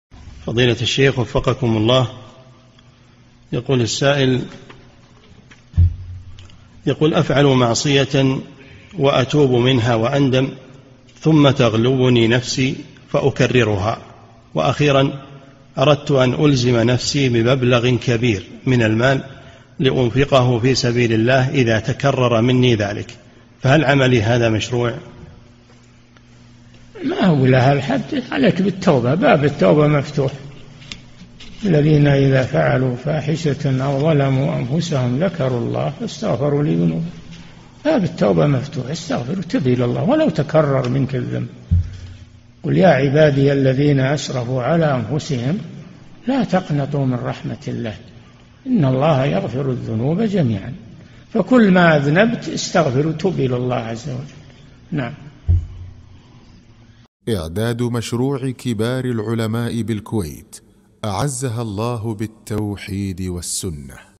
[2825 -3022] يريد أن يقلع عن معصية فألزم نفسه بمبلغ كبير من المال صدقة إذا تكررت منه هذه المعصية
-"[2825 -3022] السؤال: يقول: افعل معصيةً وأتوب منها وأندم ثم تغلبني نفسي فأكررها وأخيرا أردت أن الزم نفسي بمبلغٍ كبير من المال لأنفقه في سبيل الله إذا تكرر ذلك فهل عملي هذا مشروع؟
الجَوَابُ: ما هو إلى هذ الحد ، عليك بالتوبة، باب التوبة مفتوح، {وَالَّذِينَ إِذَا فَعَلُوا فَاحِشَةً أَوْ ظَلَمُوا أَنْفُسَهُمْ ذَكَرُوا اللَّهَ فَاسْتَغْفَرُوا لِذُنُوبِهِمْ } [آل عمران: 135] باب التوبة مفتوح، استغفر وتب إلى الله، ولو تكرر منك الذنب، {قُلْ يَاعِبَادِيَ الَّذِينَ أَسْرَفُوا عَلَى أَنْفُسِهِمْ لَا تَقْنَطُوا مِنْ رَحْمَةِ اللَّهِ إِنَّ اللَّهَ يَغْفِرُ الذُّنُوبَ جَمِيعًا} [الزمر: 53] فكل ما أذنبت استغفر وتب إلى الله عز وجل.
للاشتراك في قناة الشيخ صالح الفوزان

للاستفسار والإقتراحات والدعم
رئيس الفريق
لمشروع كبار العلماء